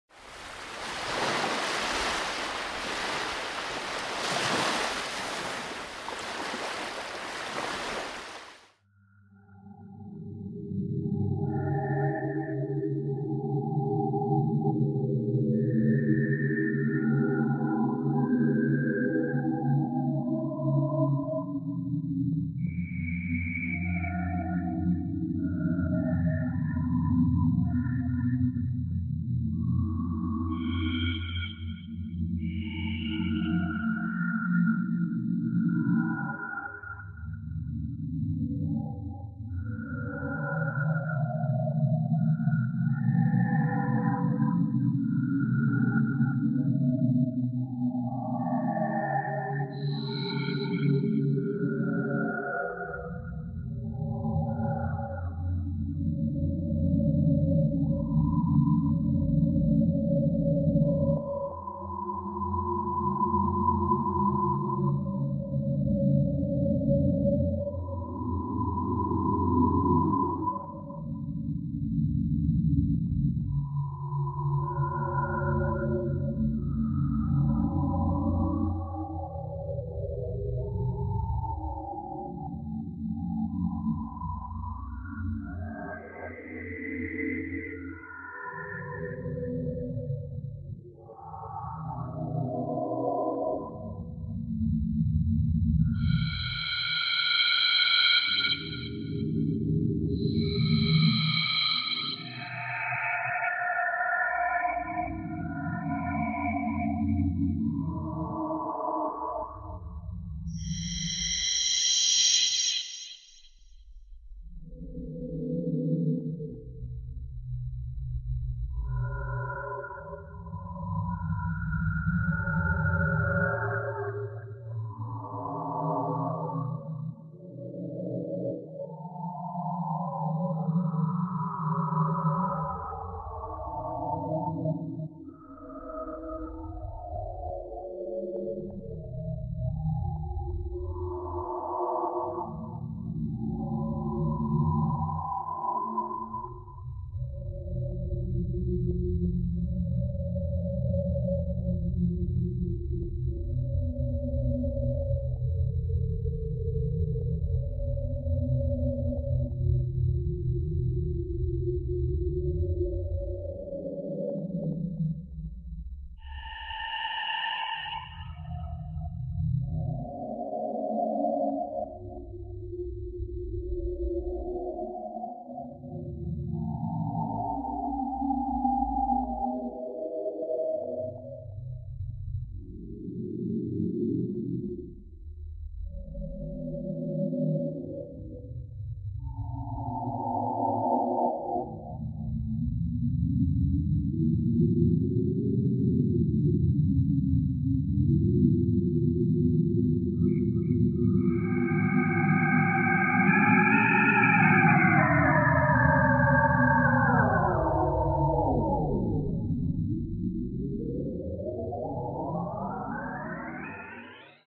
Jellyfish Invasion - Deepsea Oddities
Since the 2007 'invasion' of Nomura Jellyfish in Japan, massive surges in jellyfish blooms have destroyed fish populations and threatened people, as well as the delicate balance of marine ecology. Is this a man-made result of global warming?